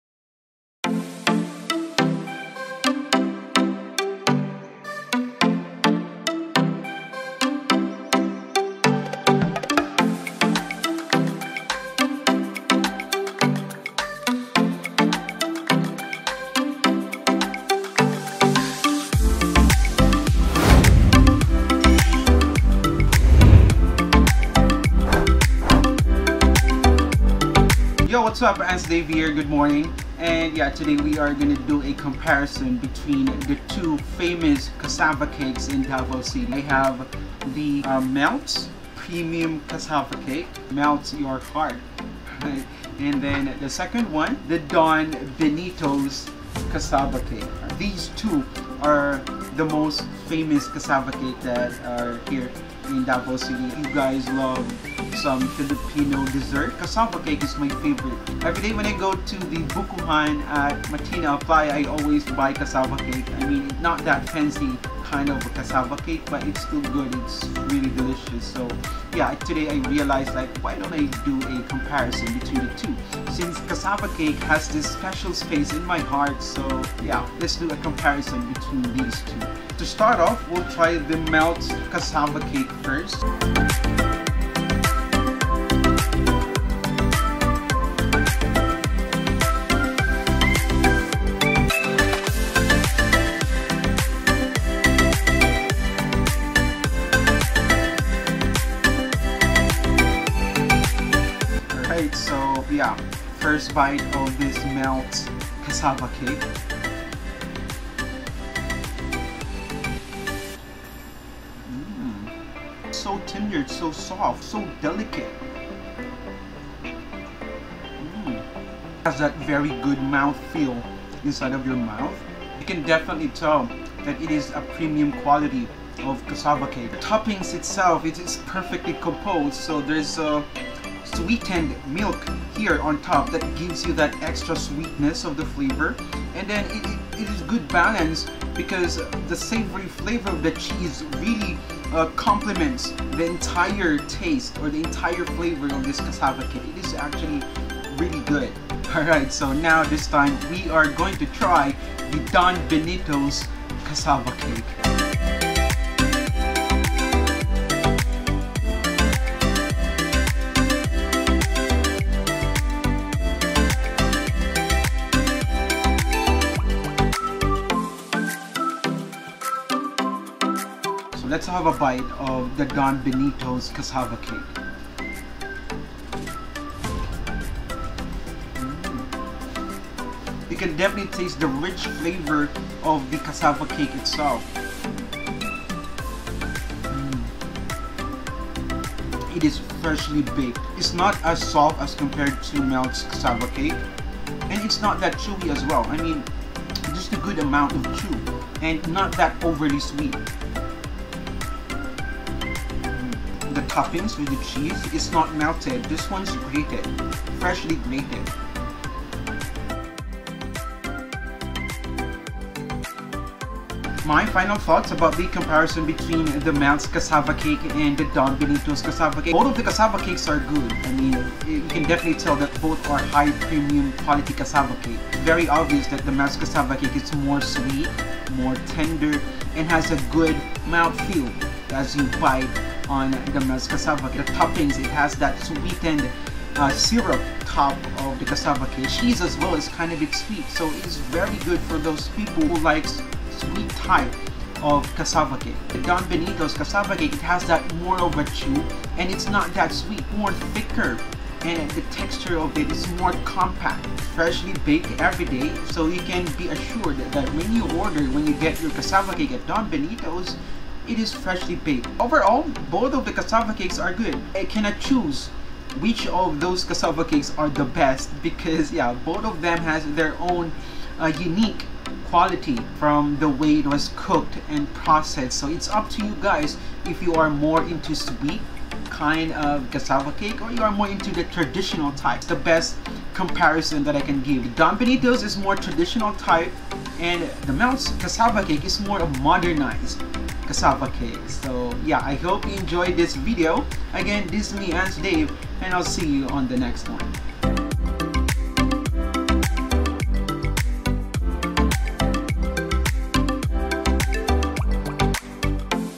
Best Cassava Cake in Davao? | Melts Cassava Cake | Don Benito's Cassava Cake | Davao Food Vlog
Melts Cassava is originally from South Cotabato and it just recently made its debut here in Davao City. Their cassava cake is really tasty, soft, melts in your mouth. Its sweetness is mainly from its sweetened topping with toasted melted cheese. It's chilled when you buy it so you know it is prepared way ahead. It comes in one size only. They are located along McArthur highway Matina fronting Malayan College.

Don Benito's, on the other hand, is available all over the country. Their cassava cakes are freshly baked everyday so its still warm when you buy it. I can say that the sweetness comes mainly from the cake itself and is topped with freshly grated cheese. They also offer Pichi-Pichi in small and big sizes. If you want to visit them, they have many branches in Davao City.

So which is which? Well, grab a Melts and Don Benito's now and taste them for your self.